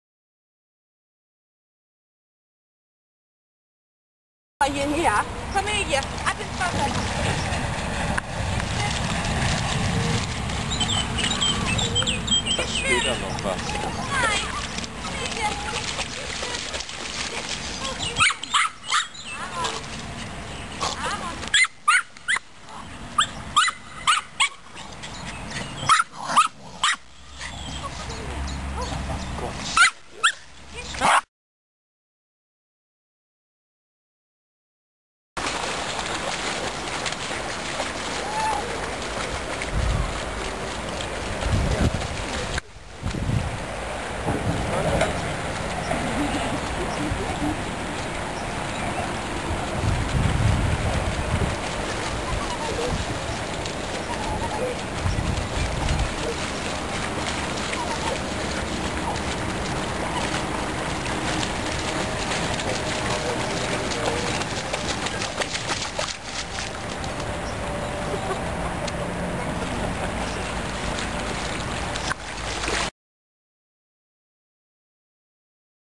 Fienchen kann auch schwimmen!
my Norfolk Terrier Aladin is a great swimmer - but my little Norfolk girl Mrs. Beasley learned it too!